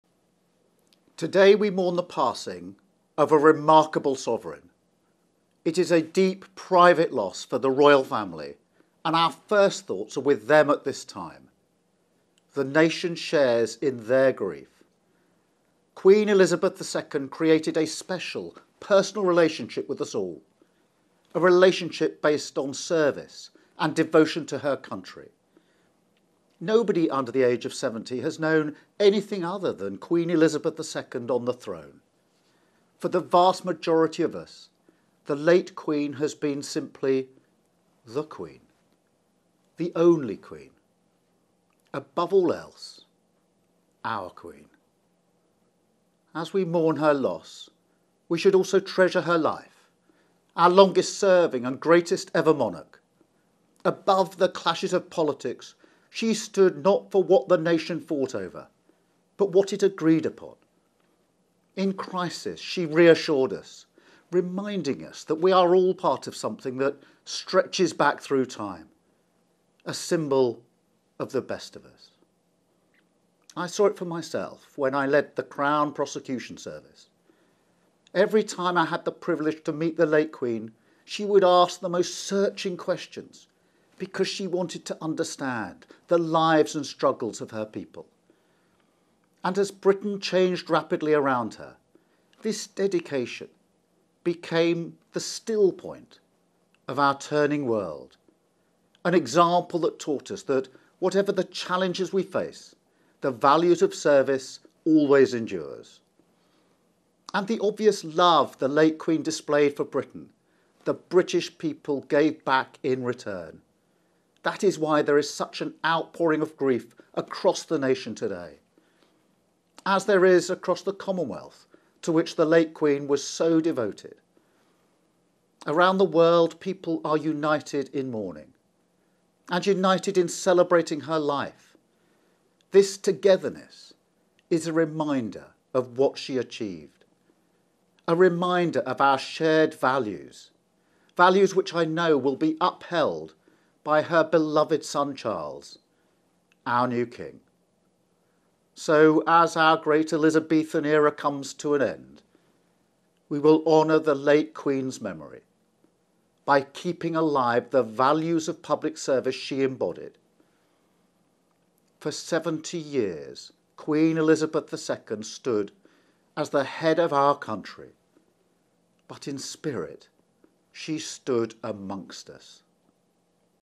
Labour leader Sir Keir Starmer pays tribute to the Queen
Labour leader Sir Keir Starmer has paid tribute to The Queen who has died at the age of 96, saying the country will "treasure her life of service".